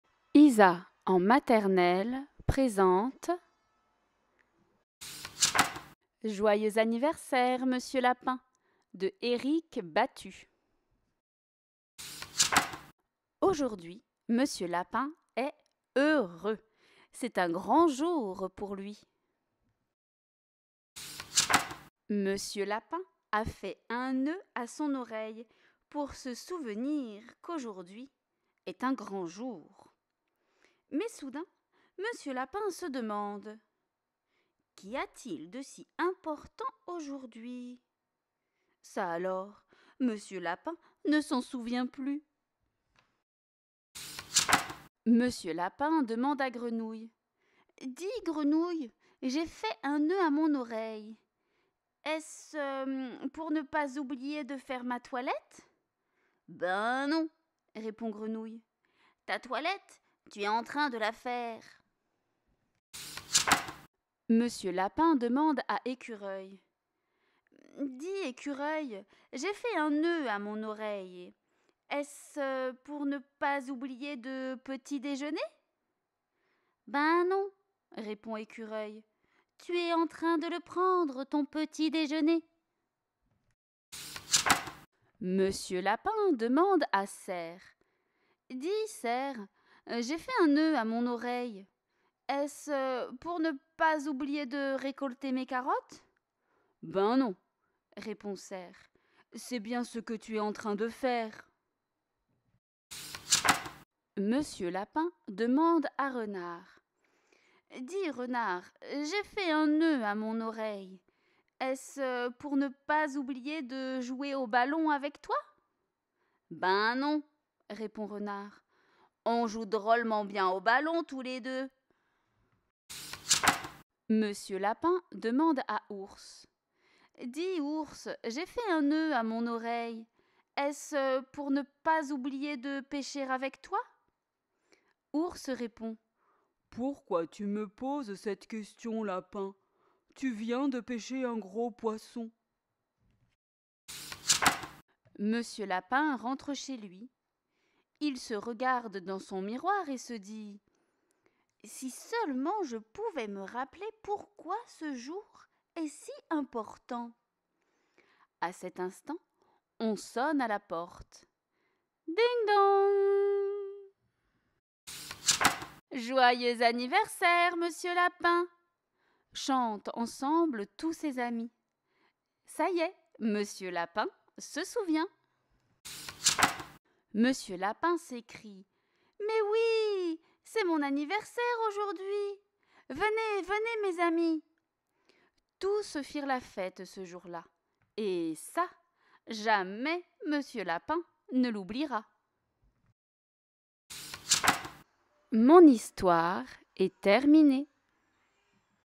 Joyeux anniversaire Monsieur Lapin, Eric Battut
Une histoire de lapin qui n'a pas de mémoire.